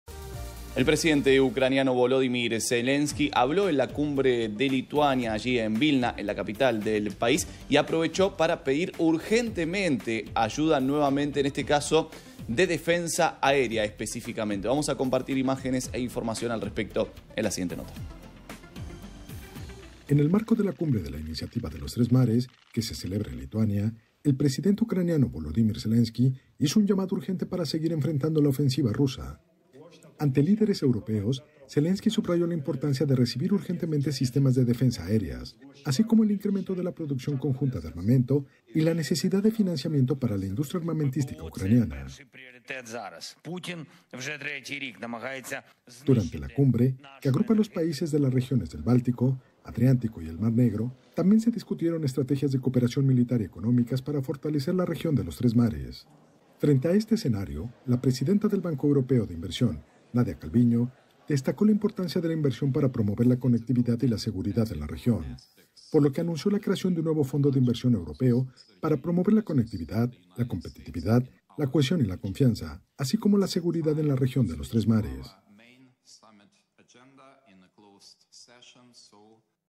GUERRA RUSIA - UCRANIA | Zelenski reclama más ayuda de Europa
👉 El presidente de Ucrania, Volodímir Zelenski, insistió de nuevo con el misma petición de auxilio a Occidente para que apoye aún más decididamente a su país en la guerra contra Rusia.